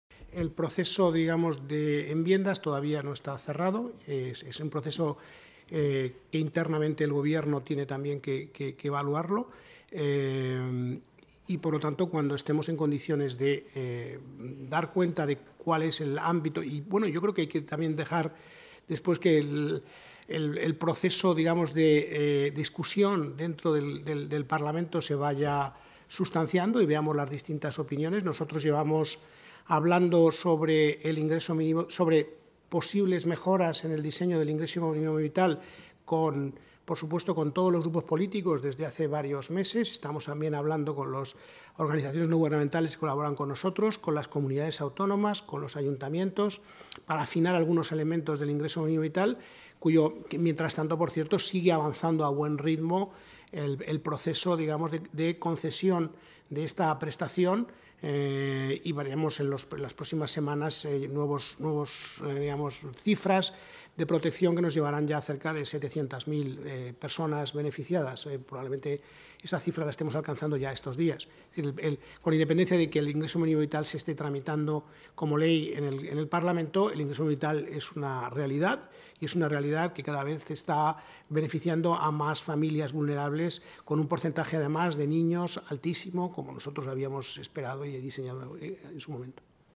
Escrivá dice que "en estos días" se alcanzarán 700.000 beneficiarios del IMV
El ministro de Inclusión, Seguridad Social y Migraciones, José Luis Escrivá, ha asegurado este jueves que "en estos días" se alcanzarán las 700.000 personas beneficiarias del Ingreso Mínimo Vital (IMV).